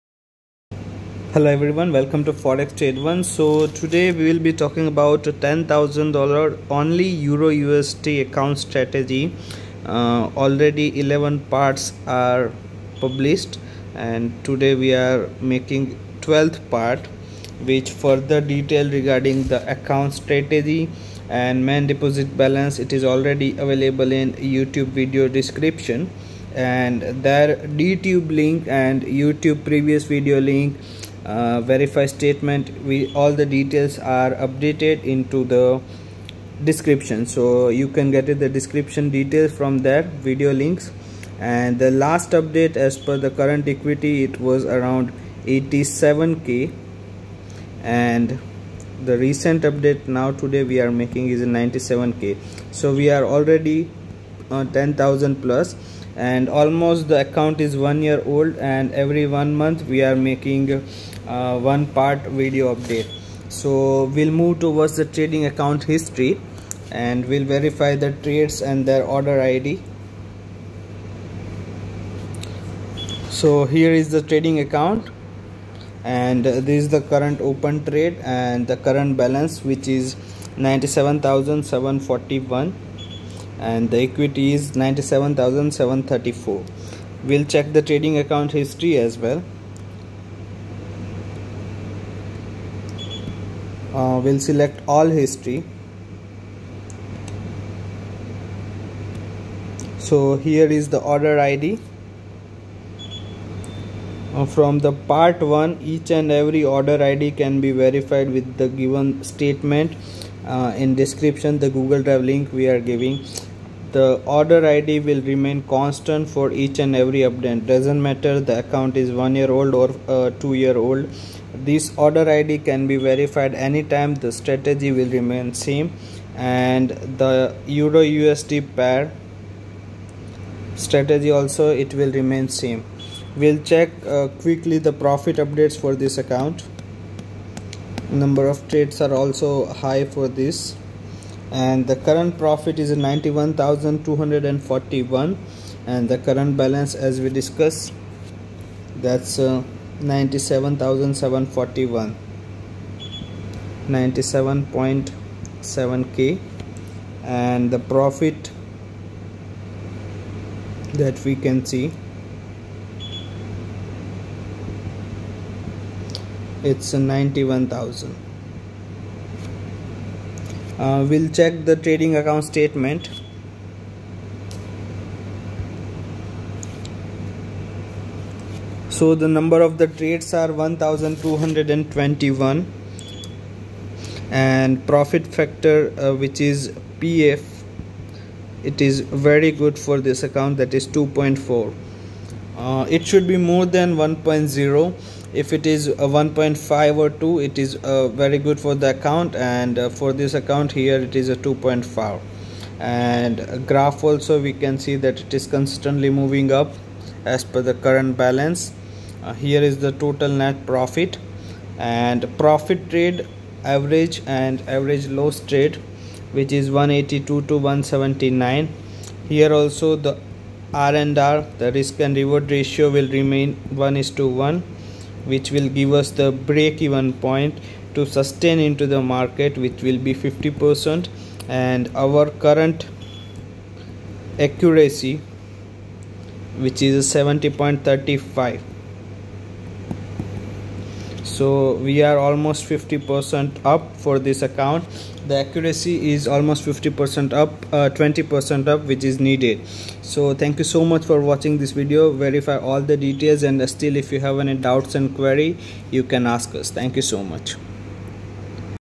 $10k Account with 910% ROI in 11 months = EURUSD ONLY - $10k-EURUSD-PART13
Chapter
00:00 Introduction
00:25 Verify Previous Update
01:10 Trading Account History
02:55 Trading Account Statement